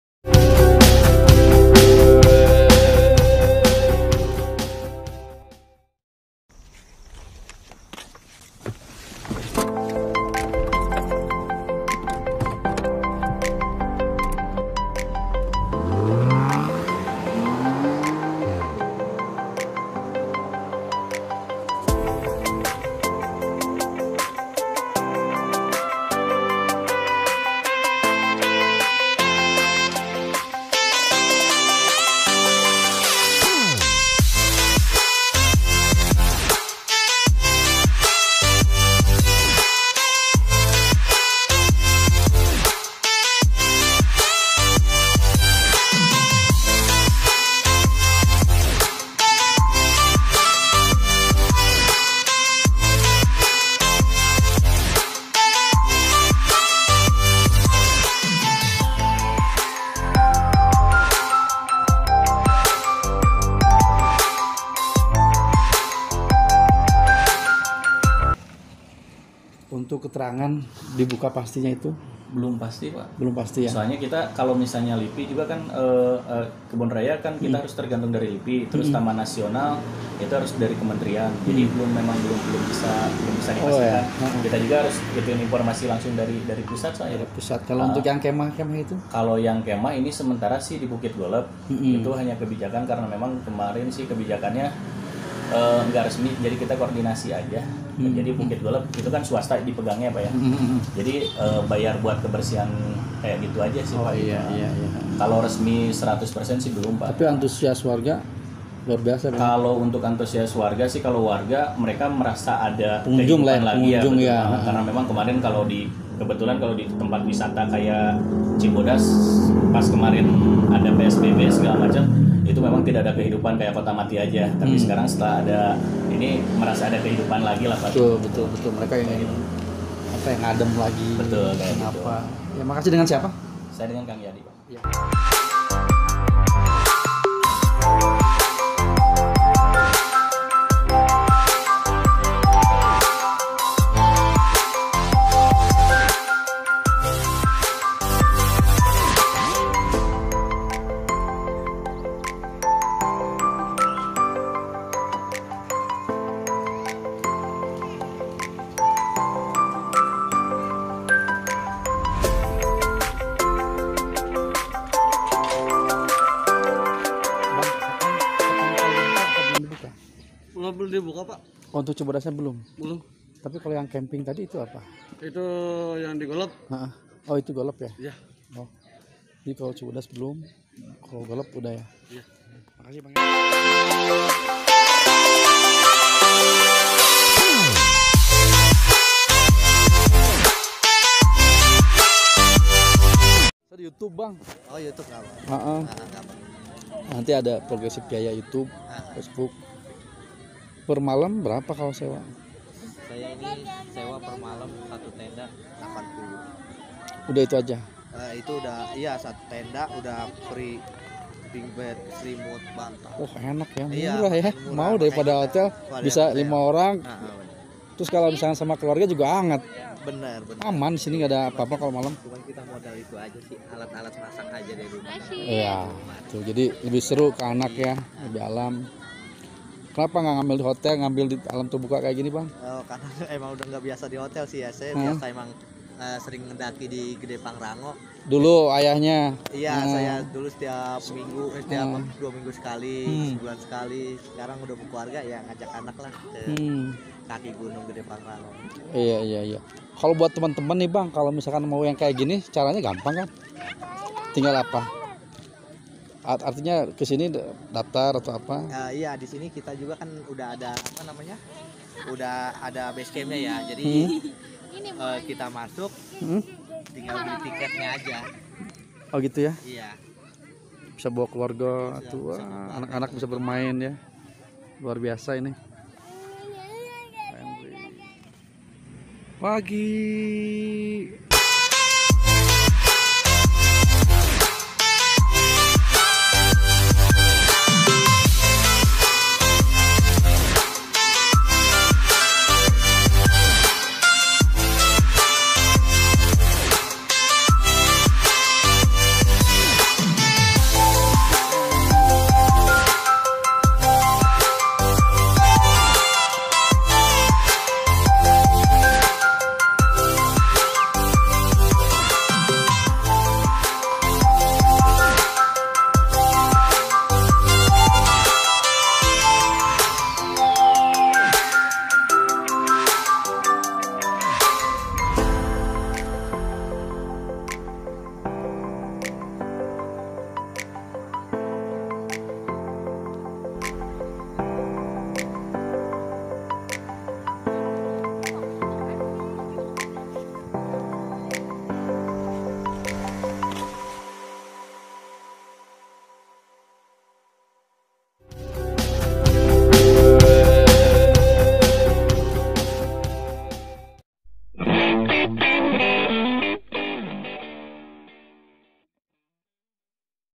Kawasan Wisata Cibodas Kembali Menggeliat
Progresif Jaya TV - Di masa transisi Pembatasan Sosial Berskala Besar (PSBB) atau menuju adaptasi kebiasaan baru (AKB) di sejumlah wilayah termasuk di Kabupaten Cianjur dibarengi dengan dibukanya kembali kawasan wisata Cibodas membuat sebagian masyarakat sangat antusias.

Setelah sekian bulan berada di rumah yang bisa membuat kondisi semakin stres, masyarakat membutuhkan adanya ruang terbuka seperti tempat wisata alam untuk menghilangkan penat.

Seperti di wilayah kawasan wisata Kebon Raya Cibodas, wisatawan lokal antusias mengunjungi kawasan tersebut, baik sekedar untuk refresing bersama keluarga maupun kerabat. Namun wisatawan hanya bisa menikmati segarnya alam karena Kebon Raya Cibodas dan Gunung Gede Pangrango masih ditutup.

Menurut salah satu karyawan pengelola Kebon Raya Cibodas, rencananya akan dibuka kembali pada tanggal 7 atau 8 Juli. Tetapi, masih menunggu konfirmasi dari pusat.

“Belum ada kepastian kapan kembalinya dibuka karena masih menunggu keputusan dari pusat,” kata Kang Yadi pada Progresif Jaya, Sabtu (4/7).

Akan tetapi, bagi beberapa wisatawan bisa menikmati sejuknya alam dengan berkemah di Bumi Perkemahan Bukit Golf Cibodas yang dikelola pihak swasta. Pantauan langsung Progresif Jaya di lapangan, ada beberapa wahana wisata di kawasan wisata Cibodas, salah satunya Bumi Perkemahan Bukit Golf Cibodas.

Salah seorang wisatawan yang berkemah, Jaka mengungkapkan, antusias dirinya bersama keluarga dapat menikmati alam dengan berkemah di Bukit Golf. Ia yang mengaku sewaktu muda sering hacking ke Gunung Gede Pangrango, setelah bekeluarga masih dapat menikmati suasana berkemah di kaki Gunung Gede Pangrango.

Mengenai biaya yang harus dikeluarkan, menurut dia, sangat terjangkau apalagi bersama keluarga.

“Per malam satu tenda 80 ribu rupiah, free (gratis) selimut, bantal, dan ini termasuk murah karena bisa lima orang. Cuman kami hanya modal alat-alat masak,” katanya pada Progresif Jaya.

Antusiasme wisatawan ini membuat kawasan wisata Cibodas kembali bergeliat. Ekonomi kembali berputar. Tetapi harus diingat, pandemi masih berlangsung dan masyarakat harus diingatkan pengelola dengan tetap mengedepankan protokol kesehatan. (Muslihat)